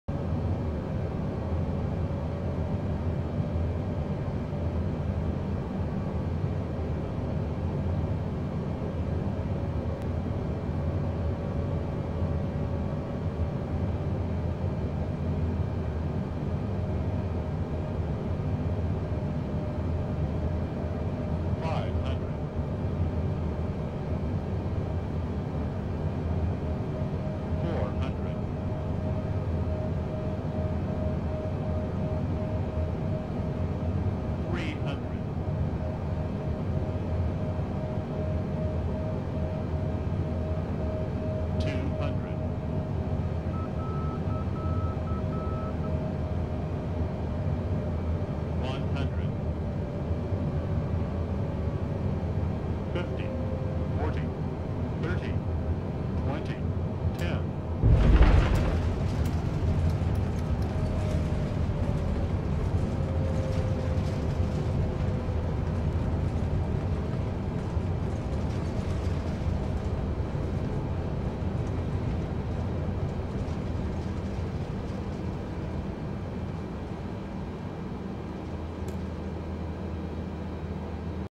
Boeing 787 Landing at Los Angeles International Airport
Los Angeles International Airport (IATA: LAX, ICAO: KLAX, FAA LID: LAX), commonly referred to as LAX (with each of its letters pronounced individually), is the primary international airport serving Los Angeles and its surrounding metropolitan area.